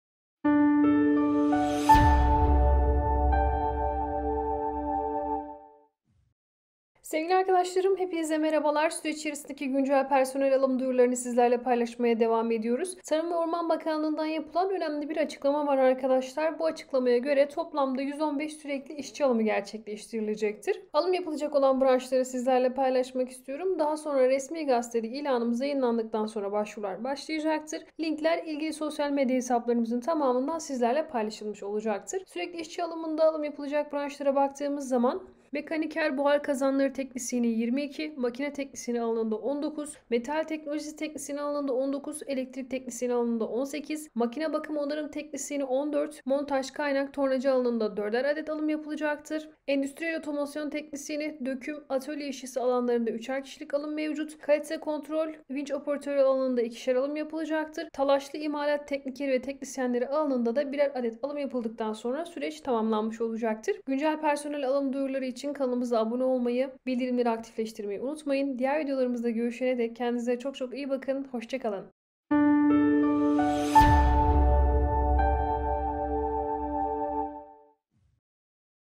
TARIM VE ORMAN BAKANLIĞI KURAYLA İŞÇİ ALIMI YAPACAK HANGİ ALANDA KAÇ ATAMA YAPILACAK AÇIKLANDI
TARIM VE ORMAN BAKANLIĞI SÜREKLİ İŞÇİ ALIMI YAPACAK- HANGİ ALANDA KAÇ ATAMA YAPILACAK AÇIKLANDI